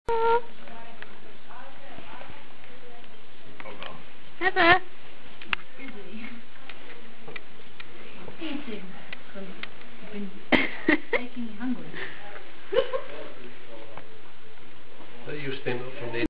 Heather in the garden
Looking in the window while we had coffee